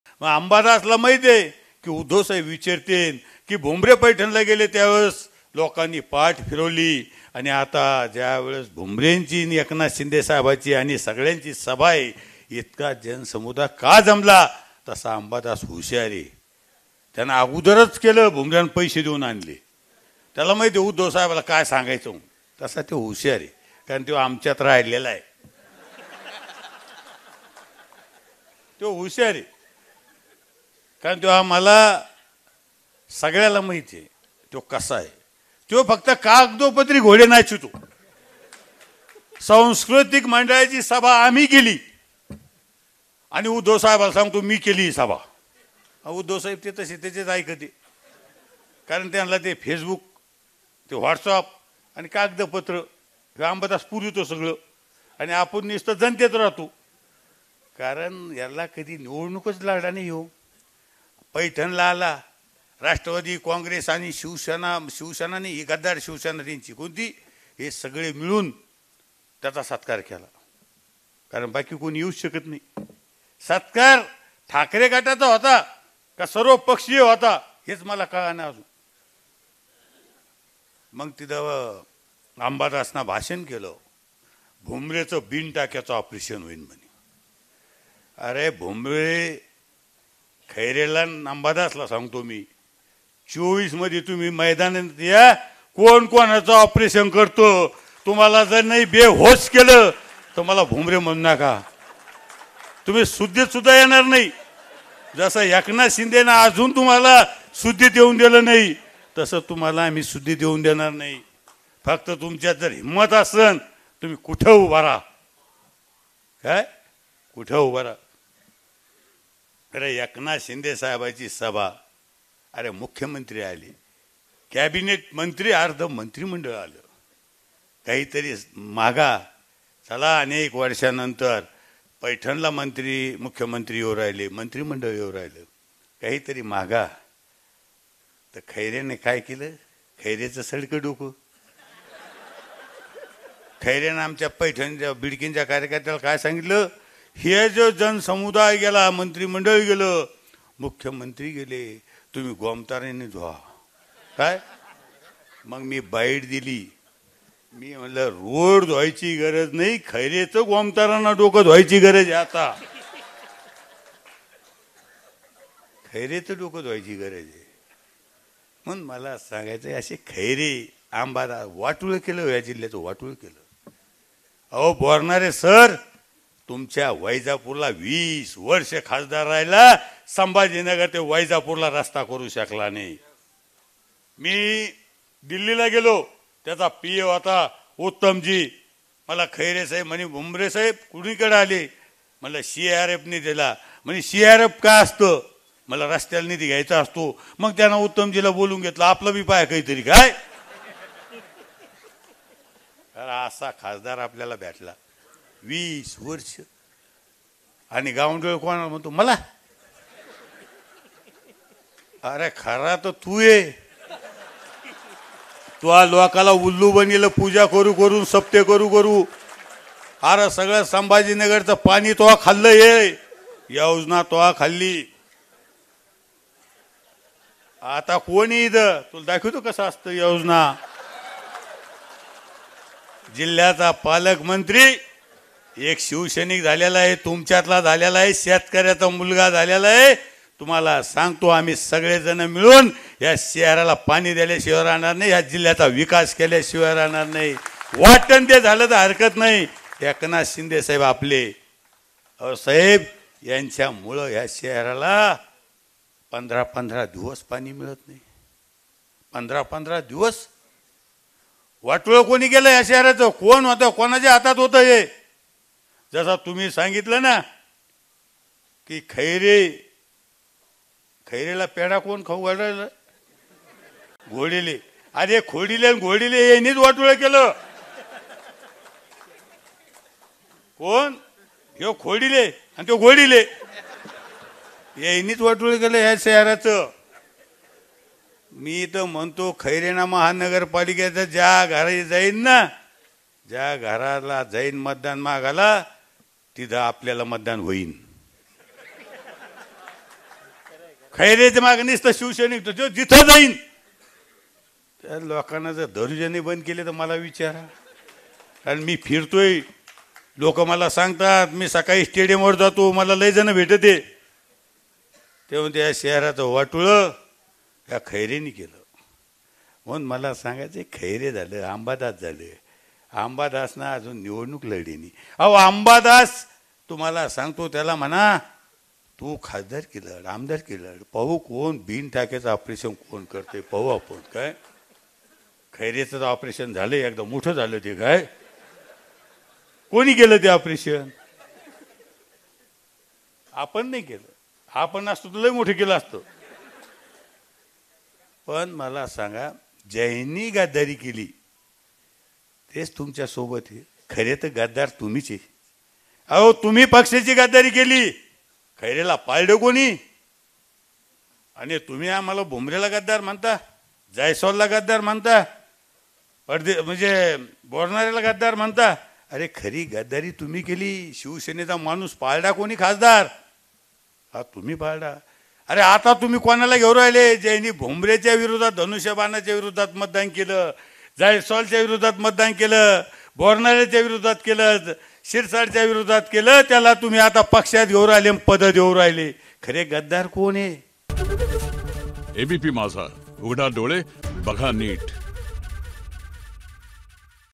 Sandipan Bhumre : संदीपान भुमरेंची तुफान फटकेबाजी, Chandrakant Khaire, Ambadas Danve निशाण्यावर
मंत्री संदीपान भुमरे यांनी आज औरंगाबादमध्ये तुफान भाषण करत विरोधकांवर निशाणा साधला. यावेळी त्यांनी उद्धव ठाकरेंपासून ते चंद्रकांत खैरे, आंबादास दानवे यांच्यावर टीका करत तुफान फटकेबाजी केली.